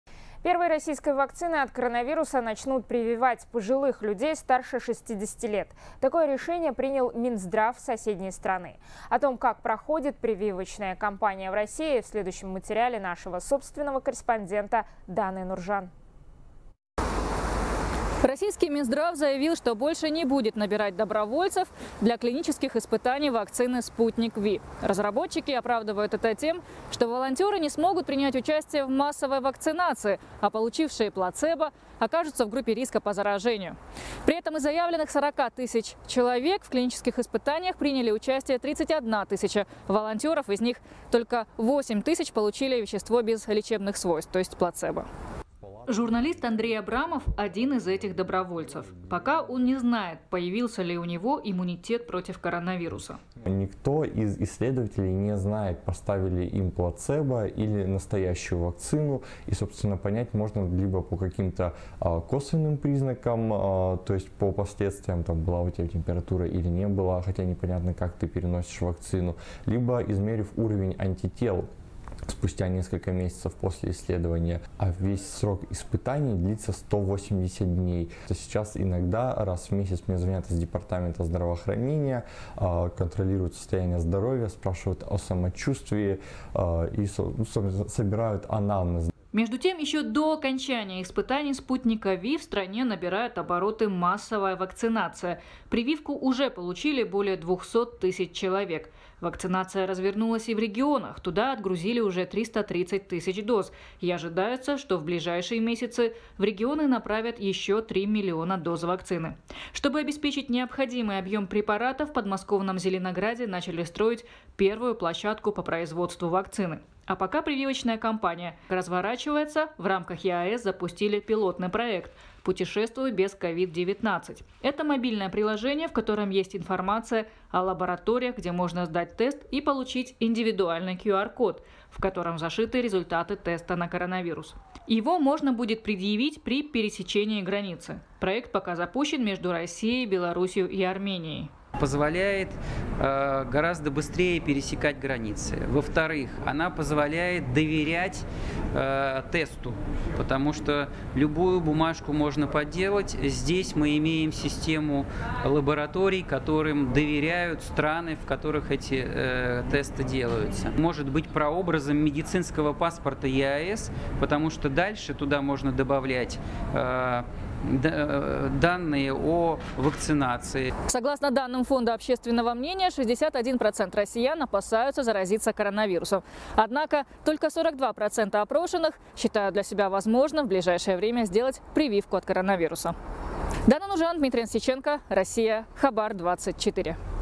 Как проходит прививочная кампания против COVID-19 в России
Первой российской вакциной от коронавируса начнут прививать пожилых людей старше 60 лет. Такое решение принял минздрав соседней страны.